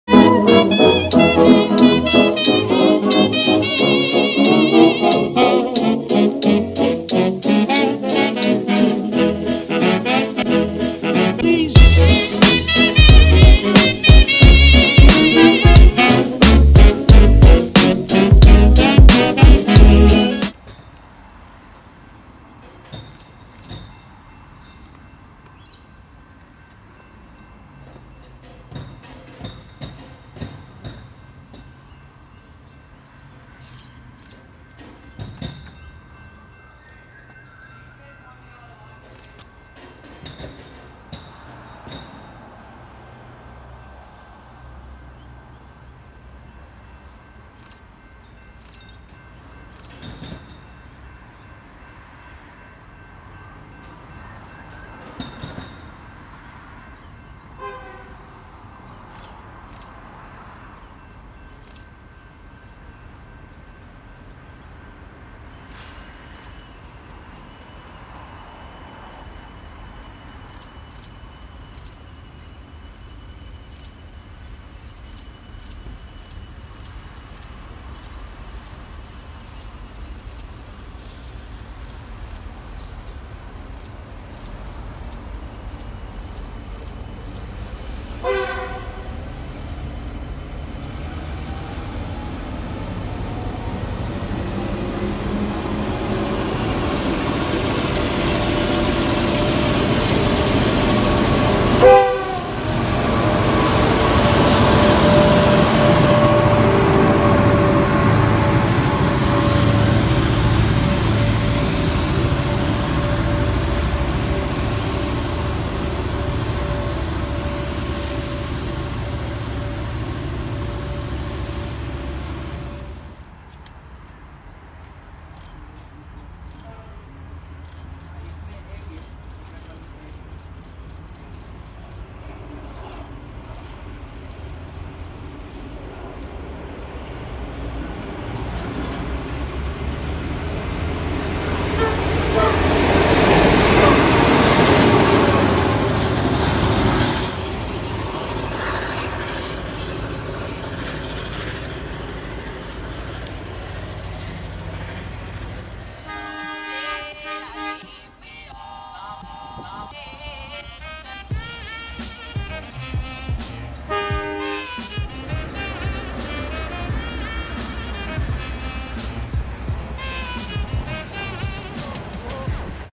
Metro’s IEV102 On The Lilydale Line!
Recently, 2 SSR T classes were tasked to haul IEV102 on an overhead wire test across Melbourne. This has been going on for some days now, and on this particular day, went to Sandringham, Werribee (on the DOWN via Altona), Glen Waverley and then went to Lilydale and back. An IEV is a rare sight anywhere, and this particular time the visit became historic, as likely the last IEV to go to the old Mooroolbark and Lilydale Stations, as they’re getting grade separated within a year, the frequency of the IEV testing on the line.

This video showcases the train at Mooroolbark on the down and Croydon on the up. Thanks to Sam’s Train Vlogs for accompanying me at both stations.

Filmed 24/05/20